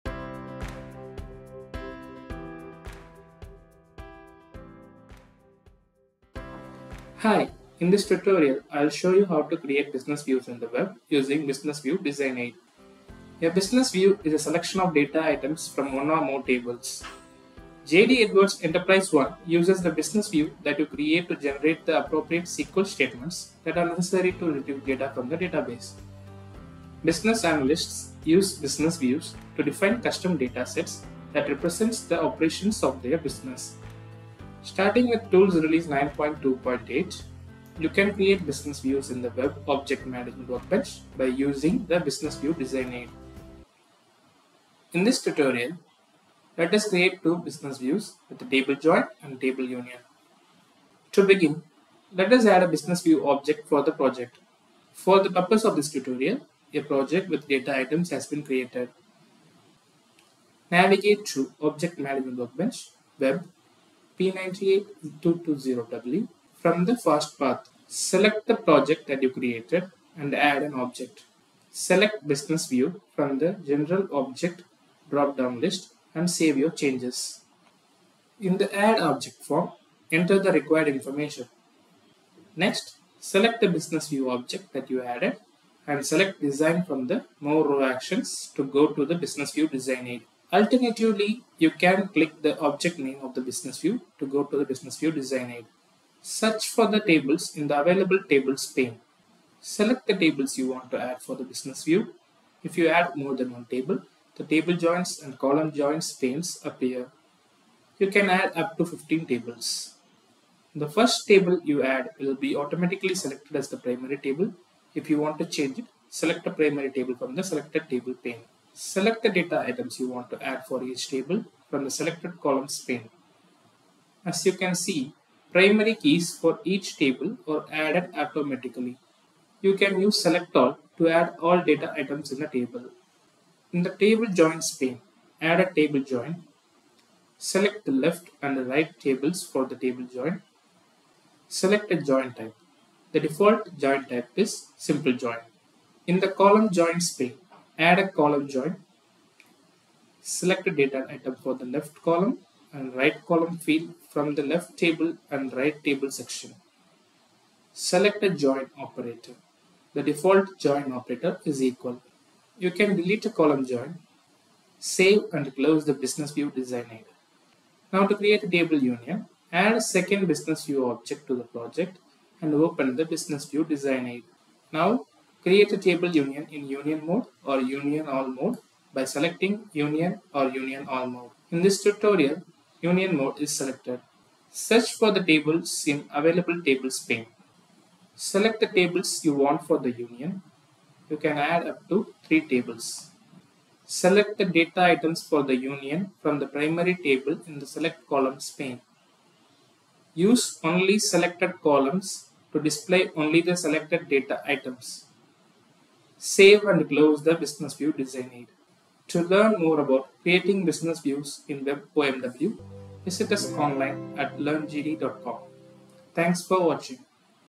Creating Business Views in Web Object Management Workbench
This tutorial shows you how to create Business Views using Business View Design Aid in the Web Object Management Workbench (Web OMW).